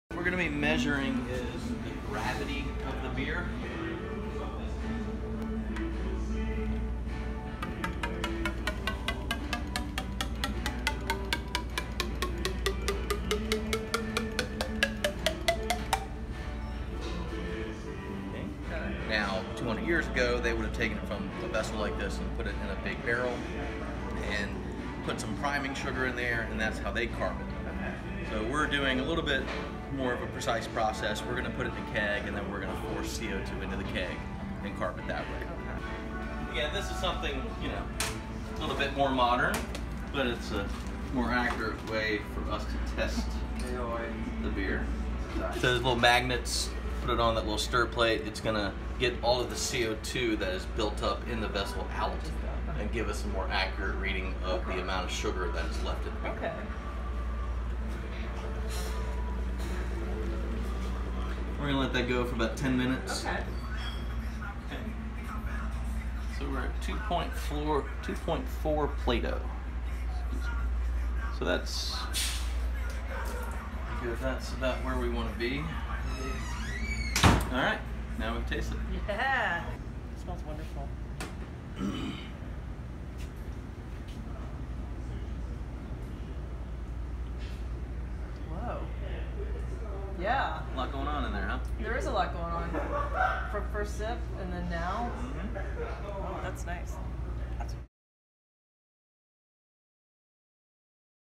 BrewHaHa 2018 - Behind the Scenes with Ardent
In the spirit of Virginia Craft Beer Month, the Virginia Museum of History & Culture will host its second annual "BrewHaHa" craft beer festival, featuring selections from 10 Virginia breweries and 2 Virginia cideries, live music, food, and more. We're also introducing a new VIP event, "Battle of the Brews" before the main festival, that showcases Richmond-area breweries' renditions of a historic beer recipe from our collection. Ardent Craft Ales is one of those Richmond breweries. Enjoy a glimpse behind the scenes at Ardent with brewer Danny Fain as he checks the gravity on their historic brew revamp.